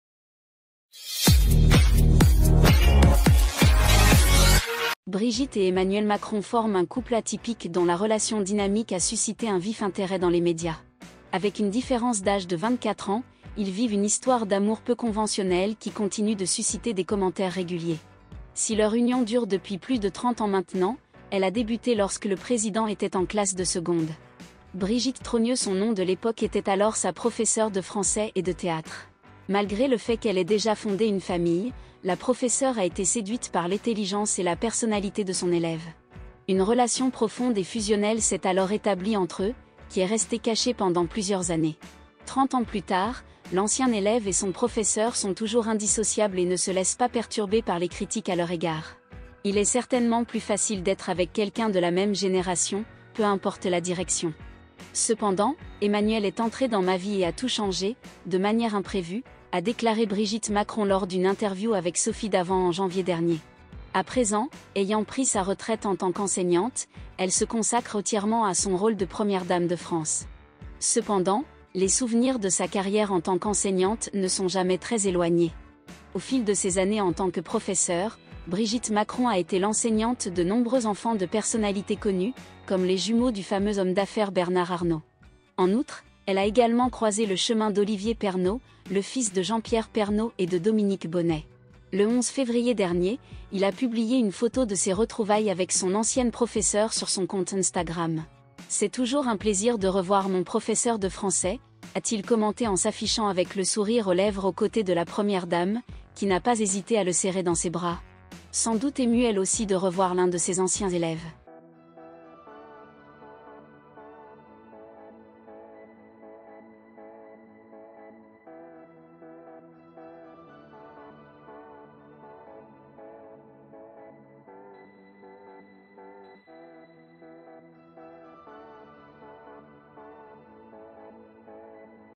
Brigitte Macron pose avec un de ses ancien élèves, un visage pas méconnu du public
Brigitte et Emmanuel Macron forment un couple atypique dont la relation dynamique a suscité un vif intérêt dans les médias. Avec une différence d'âge de 24 ans, ils vivent une histoire d'amour peu conventionnelle qui continue de susciter des commentaires réguliers.

Si leur union dure depuis plus de 30 ans maintenant, elle a débuté lorsque le Président était en classe de seconde : Brigitte Trogneux son nom de l'époque était alors sa professeure de français et de théâtre.

Malgré le fait qu'elle ait déjà fondé une famille, la professeure a été séduite par l'intelligence et la personnalité de son élève. Une relation profonde et fusionnelle s'est alors établie entre eux, qui est restée cachée pendant plusieurs années. Trente ans plus tard, l'ancien élève et son professeur sont toujours indissociables et ne se laissent pas perturber par les critiques à leur égard.

"Il est certainement plus facile d'être avec quelqu'un de la même génération, peu importe la direction. Cependant, Emmanuel est entré dans ma vie et a tout changé, de manière imprévue", a déclaré Brigitte Macron lors d'une interview avec Sophie Davant en janvier dernier. À présent, ayant pris sa retraite en tant qu'enseignante, elle se consacre entièrement à son rôle de Première dame de France.

Cependant, les souvenirs de sa carrière en tant qu'enseignante ne sont jamais très éloignés. Au fil de ses années en tant que professeur, Brigitte Macron a été l'enseignante de nombreux enfants de personnalités connues, comme les jumeaux du fameux homme d'affaires Bernard Arnault.

Brigitte Macron pose avec un ancien élève, un visage pas méconnu du public

En outre, elle a également croisé le chemin d'Olivier Pernaut, le fils de Jean-Pierre Pernaut et de Dominique Bonnet. Le 11 février dernier, il a publié une photo de ses retrouvailles avec son ancienne professeure sur son compte Instagram.